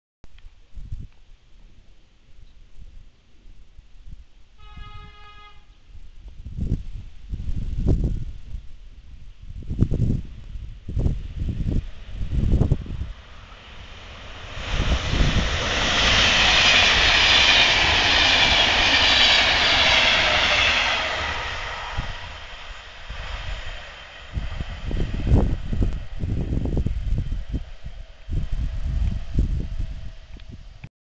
5 370 004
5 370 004 z pociągiem EIC 54 "Berlin - Gdynia Express" (Gdynia Główna - Berlin Hbf).